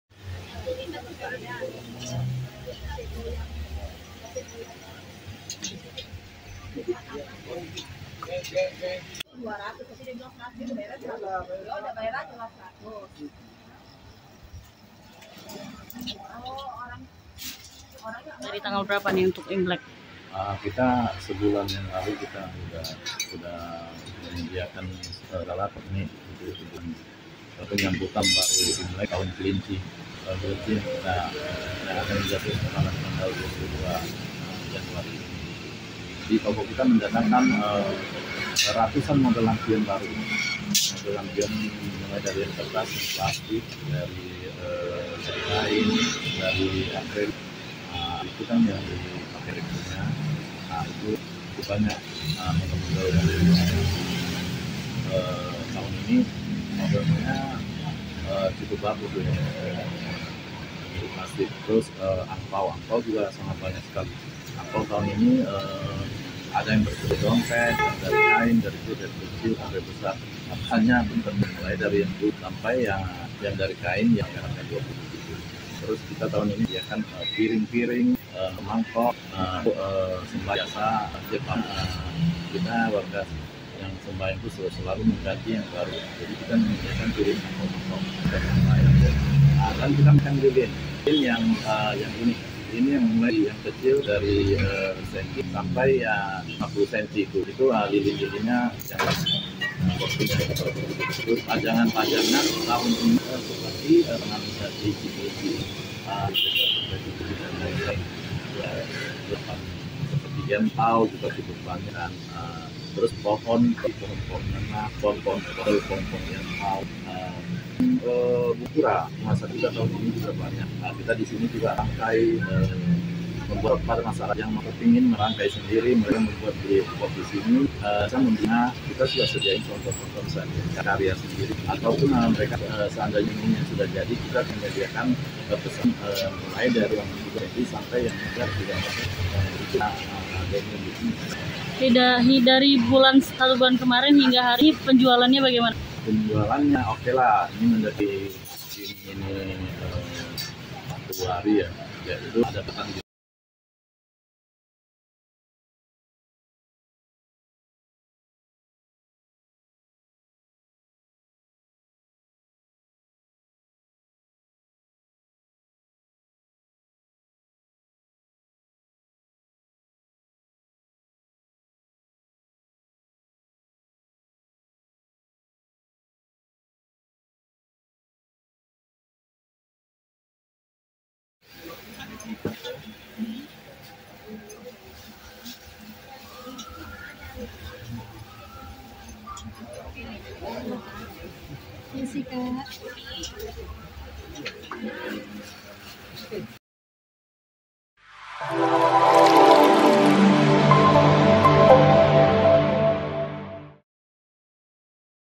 Jelang Imlek, Toko Pernak-pernik di Medan Mulai Diserbu Warga
TRIBUN-MEDAN.COM,MEDAN- Menjelang perayaan Tahun Baru Imlek pada 22 Januari 2023 mendatang, sejumlah toko penjual pernak-pernik di Kota Medan mulai diserbu masyarakat.

Satu diantara toko penjual pernak-pernik yang didatangi warga adalah Toko Acai Jaya di Jalan Brigjend Katamso Nomor 70-72, Kecamatan Medan Maimun, Kota Medan.

Pantauan Tribun Medan, beragam pernak-pernik, mulai dari lampion, angpao, stiker, gantungan, boneka dan barang lainnya terpajang rapi di etalase toko.

Terlihat pula sejumlah masyarakat silih berganti berdatangan melihat dan membeli keperluan Imlek.

Pemilik toko Acai Jaya, Aliansyah mengatakan, bawah dirinya sudah mempersiapkan berbagai pernak-pernik penyambutan tahun baru Imlek sejak satu bulan yang lalu.

"Kita satu bulan yang lalu sudah menyediakan segala pernak-pernik untuk kebutuhan penyambutan tahun baru imlek atau tahun kelinci yang akan jatuh pada 22 Januari," ujarnya kepada Tribun Medan, Senin (9/1/2023).